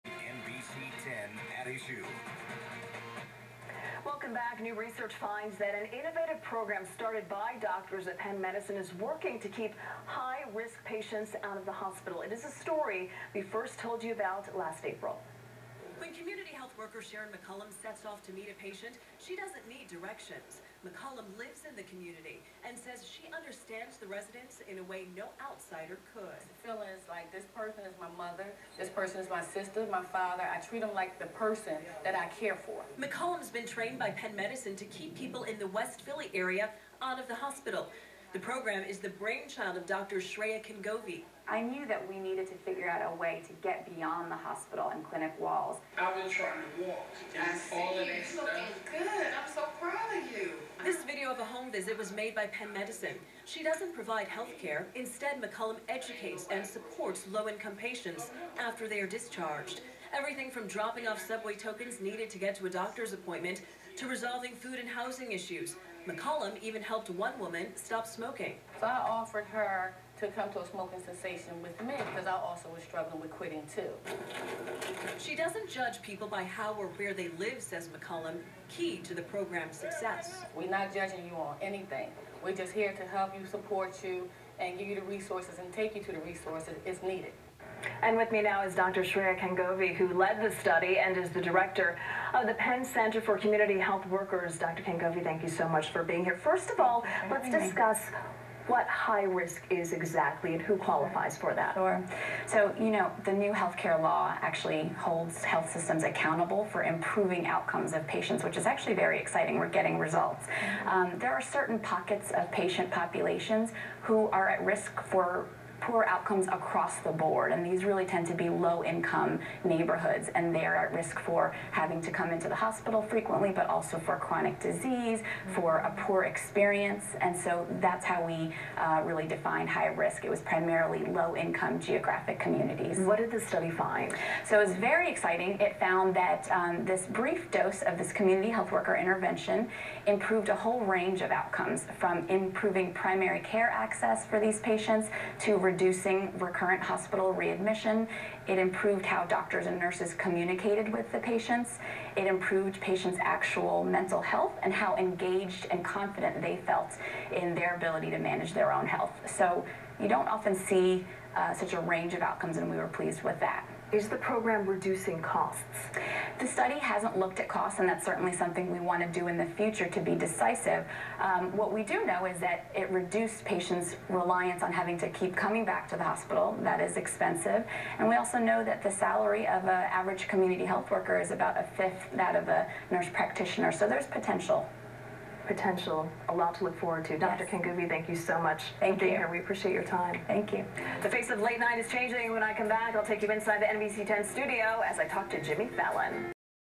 Dr.Shreya Kangovi
Dr.Shreya Kangovi, Director of Penn Center for community  Health workers talks to NBC about keeping high risk patients out of the hospital. The innovative program is the brain child of Dr. Shreya Kangovi.